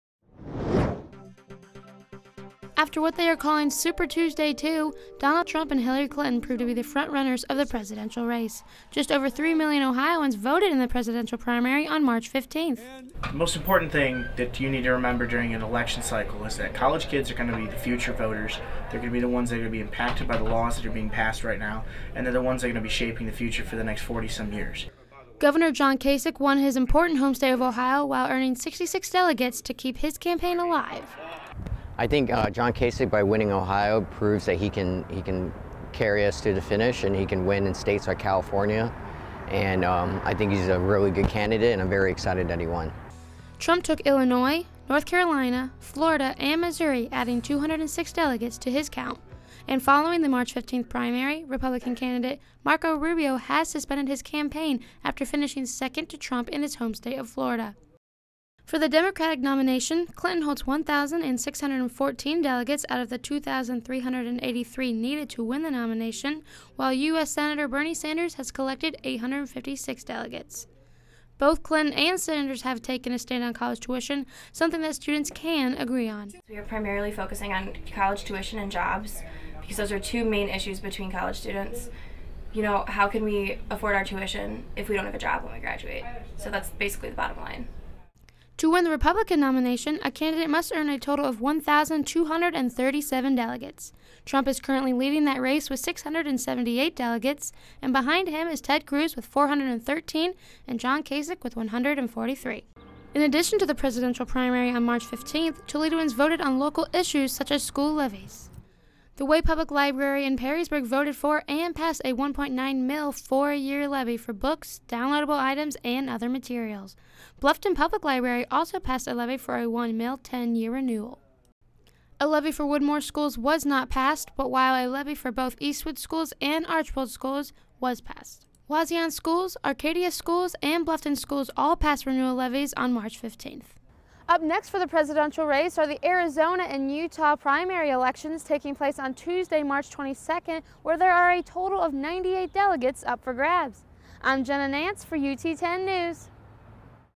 UT:10 News – Campaign Recap
Reporter Jenna Nance looks into the results from last Tuesday's primary election.